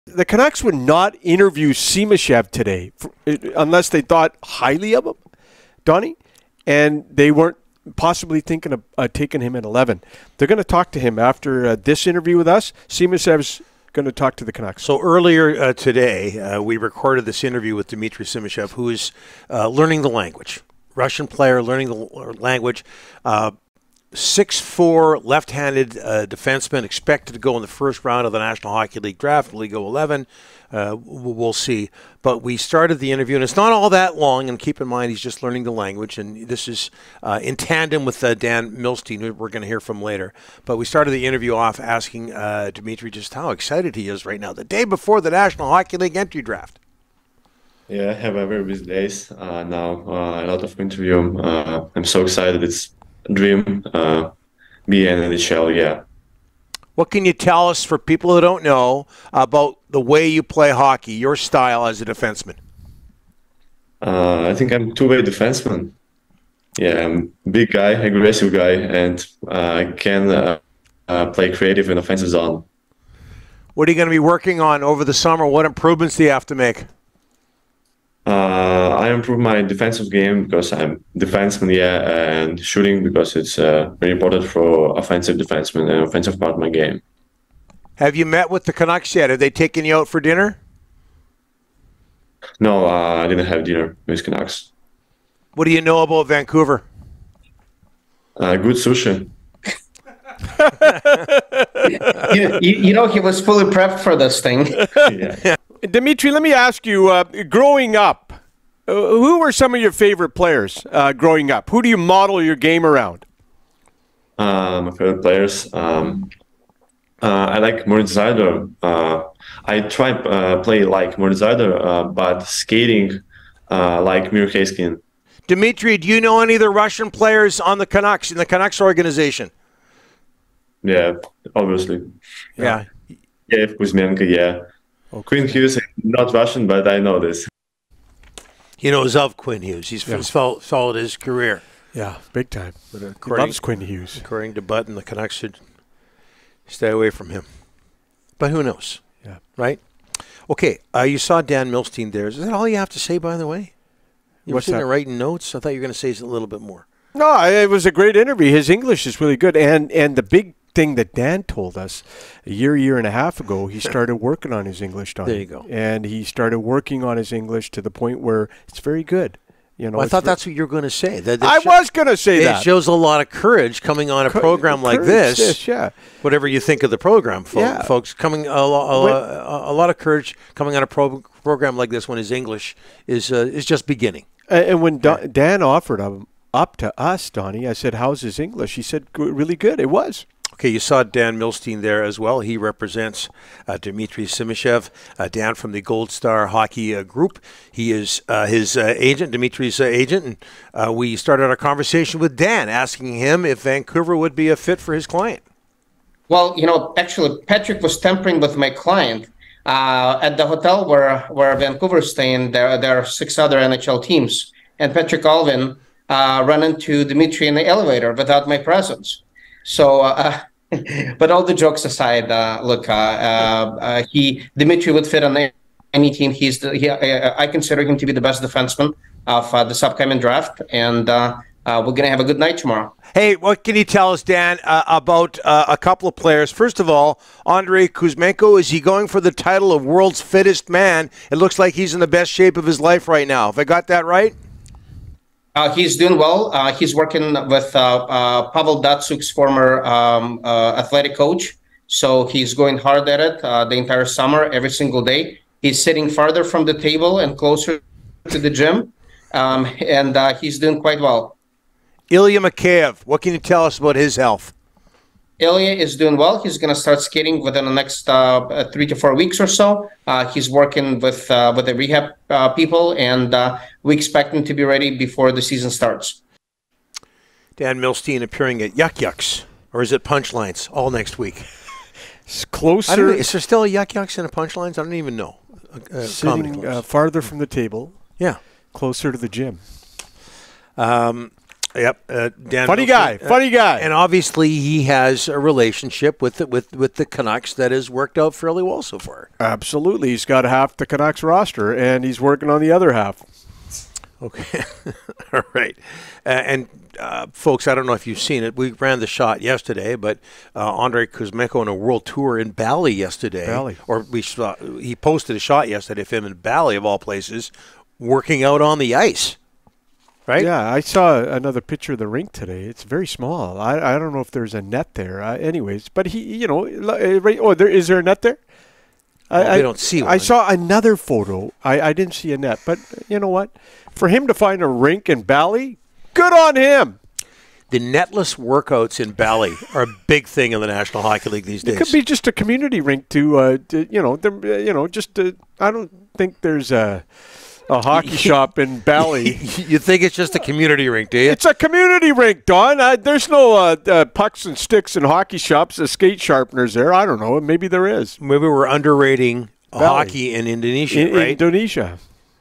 Dmitri Simashev and Dan Milstein on the NHL draft and Canucks updates
NHL prospect Dmitri Simashev and Dan Milstein joined the guys from Nashville to look ahead to the Draft tomorrow and Dan updated the latest with Kuzmenko and Mikheyev.